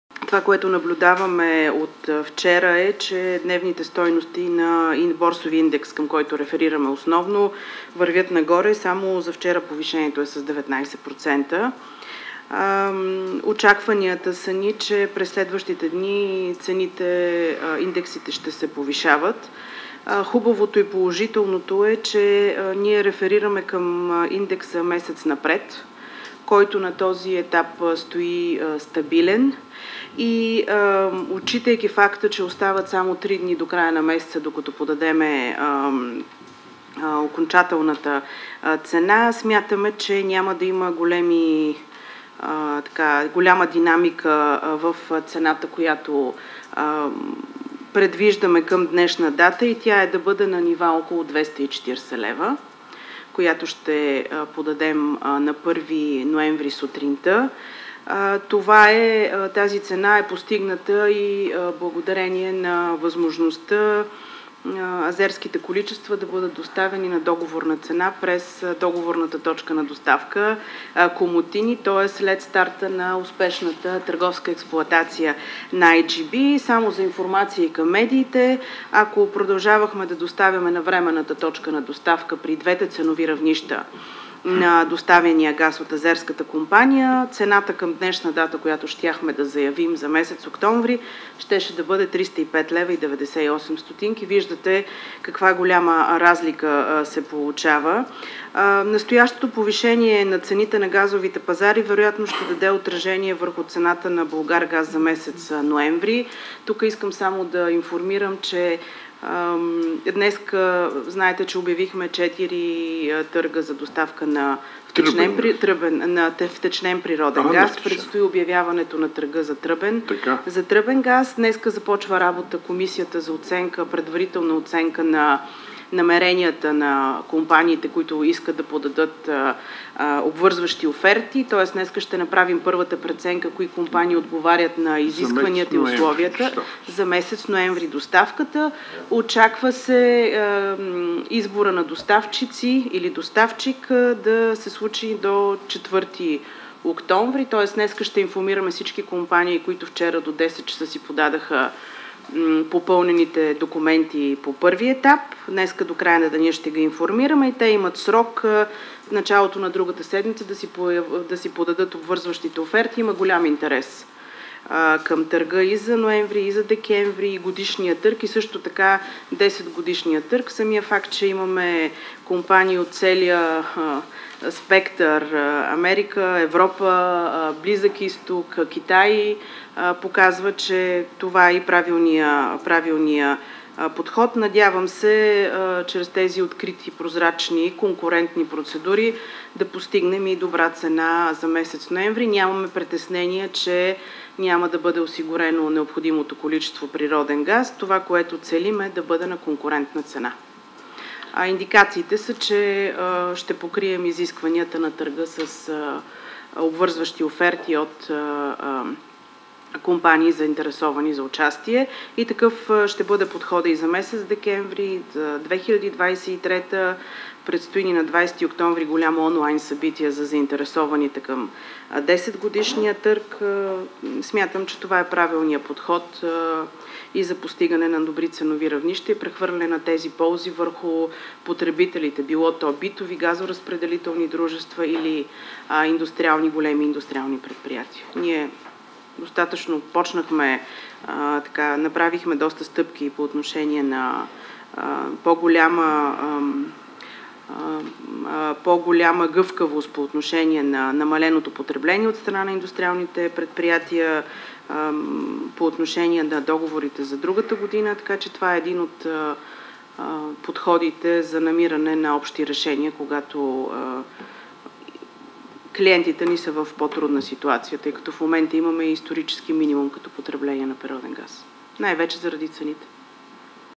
Природният газ може да поевтинее с 32 на сто следващия месец
Деница Златева обяви, че има голям интерес към обявените 4 търга за доставка на втечнен природен газ, както да краткосрочен, така и за 10-годишен период.